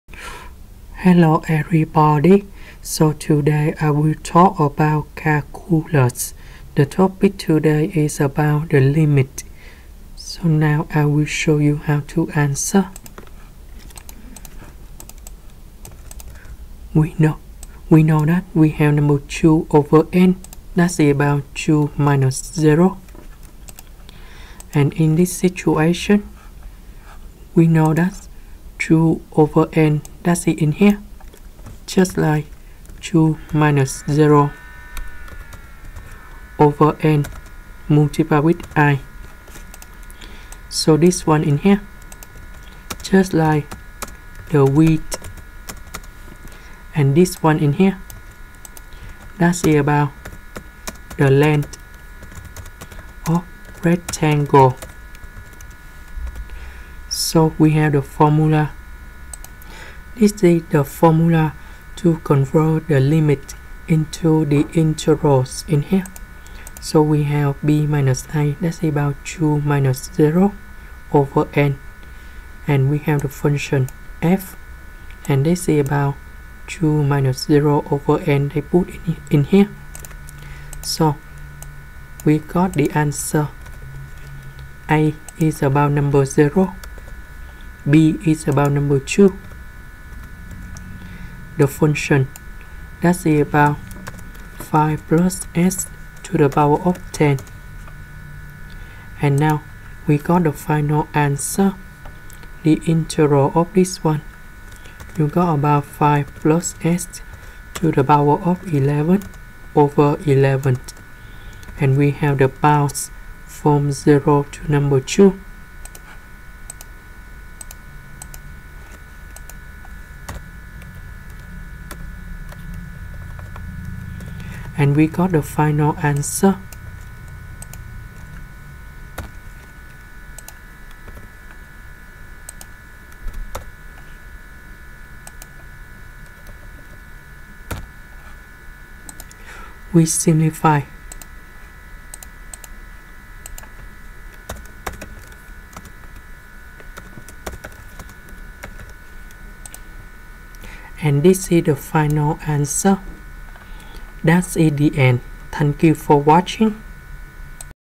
Calculus Help: Integration and Limit Combine: Lim (n→∞) ⁡∑ from i=1 to n 2/n (5+2i/n)^10
Here is the technique to solve this limit and how to find them in here